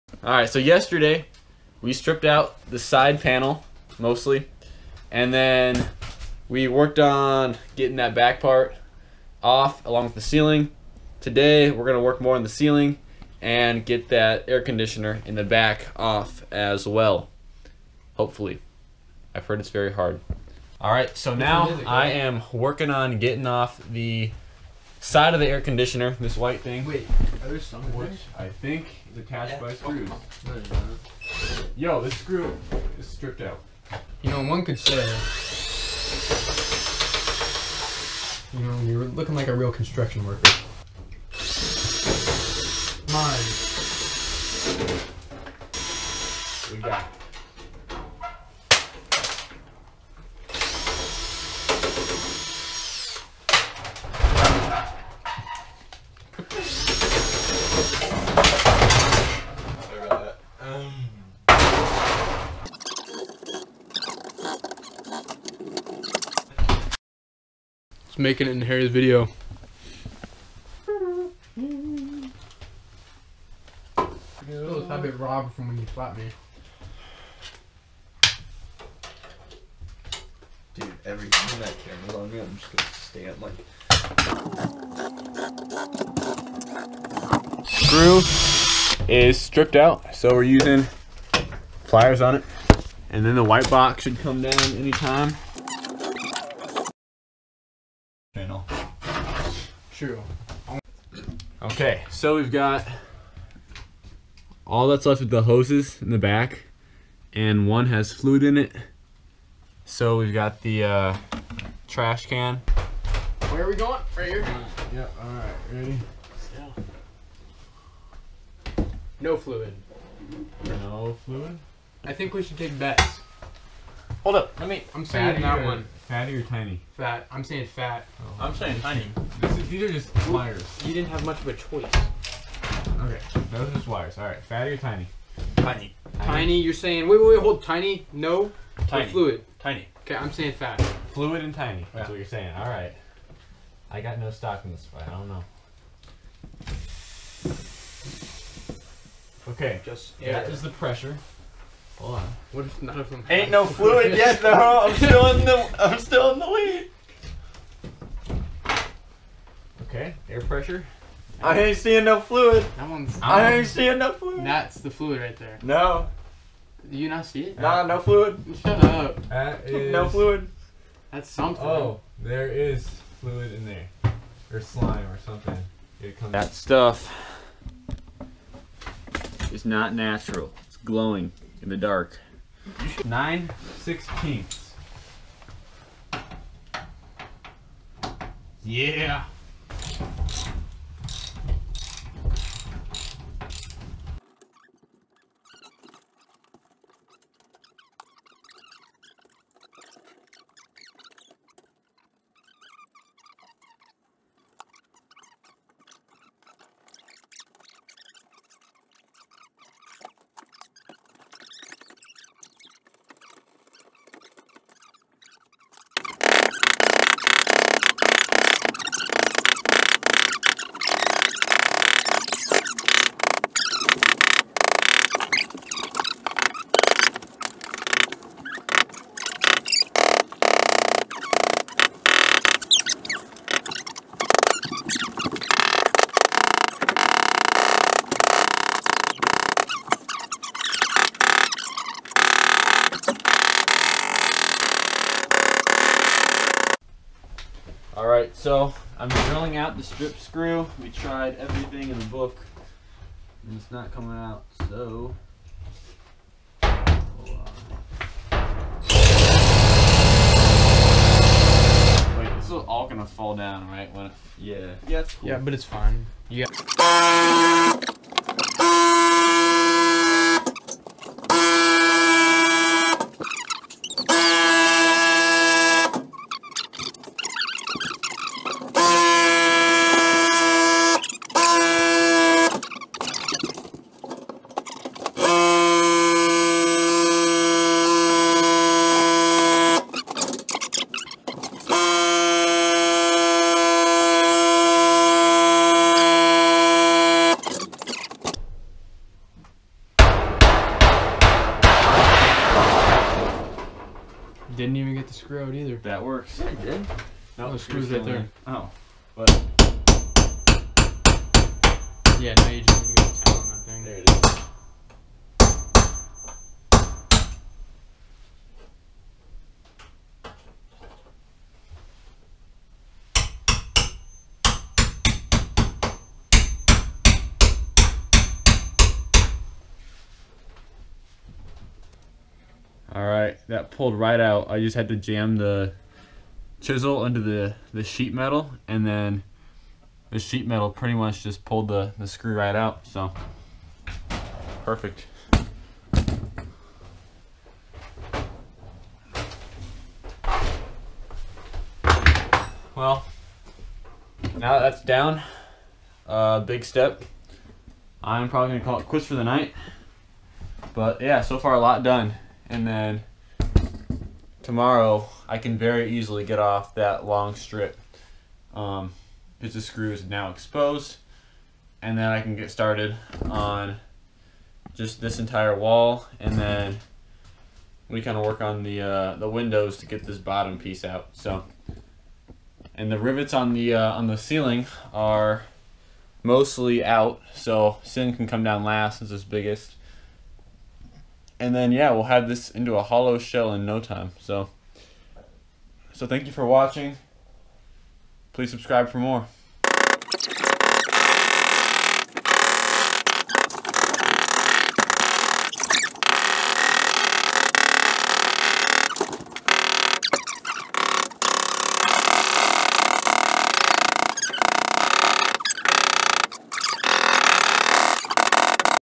Mini-Bus Conversion: Removing Rear AC and Back Wall
Today we removed the rear AC unit so we could get the sheet metal off the back wall. The back wall is what really needed to be removed first, as it held together the ceiling and side walls.